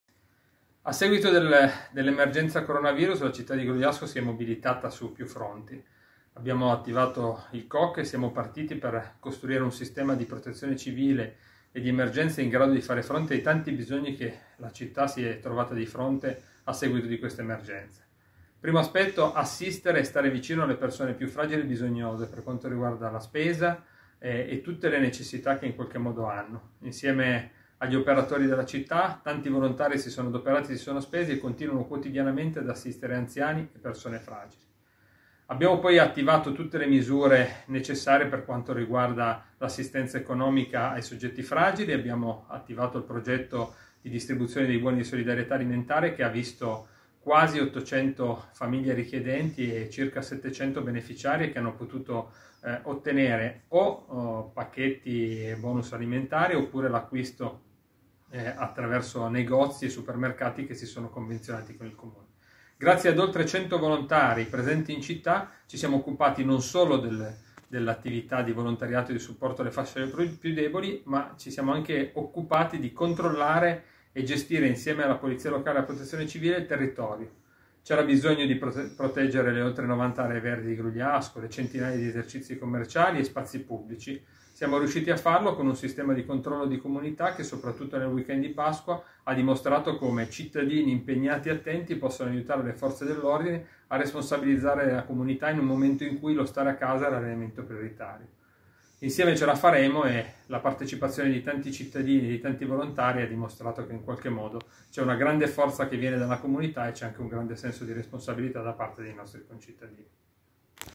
Emergenza, i sindaci protagonisti: intervista a Roberto Montà
La priorità del sindaco di Grugliasco in questa emergenza è stata quella di stare vicino alle fasce deboli e agli anziani. Il comune di Grugliasco ha dato anche assistenza economica. grazie ai buoni di solidarietà a 700 famiglie. Importante è stato anche il controllo territorio, delle aree verdi e degli esercizi commerciali effettuato da 100 volontari.
Il sindaco ringrazia  volontari, operatori e cittadini per la grande forza di comutà dimostrata e il grande senso di responsabilità.